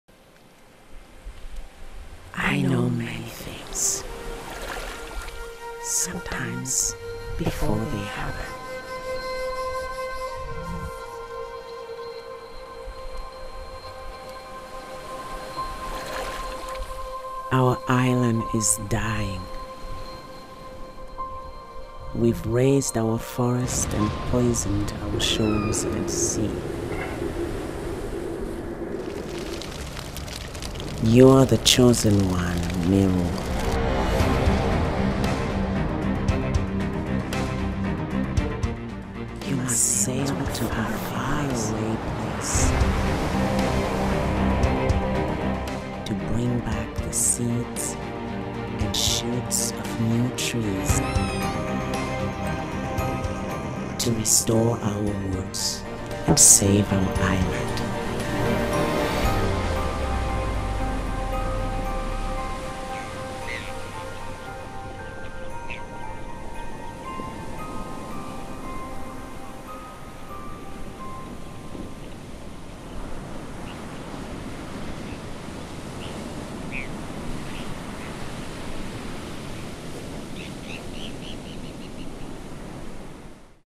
Wide as the Wind by Edward Stanton - Book Trailer
he lyrical tale of a boy, a girl, their island, and how they saved it.

Wide as the Wind is the first novel to deal with the stunning, tragic history of Easter Island (Vaitéa). It could be described as quest fiction for all ages in the line of Tolkien’s The Hobbit, but it is set in the real world, not Middle-earth. Wide as the Wind portrays Polynesian voyages across the Pacific Ocean in canoes with no metal parts or instruments: the greatest adventure in human prehistory, as bold as modern space voyages (National Geographic).

When Vaitéa is ravaged by war, hunger and destruction, it falls upon Miru, the fifteen-year-old son of a tribal warrior, to sail to a distant island to find the seeds and shoots of trees that could reforest their homeland. If he decides to undertake the voyage, he must leave behind Kenetéa, a young woman from an enemy tribe with whom he has fallen deeply in love. And if Miru and his crew survive the storms, sharks and marauding ships that await them on a journey over uncharted ocean, an even greater mission would lie ahead. They must show their people that devotion to the earth and sea can be as strong as war and hatred. Wide as the Wind is both a stirring novel of adventure and a prophetic tale for our times.

and your favorite bookstore.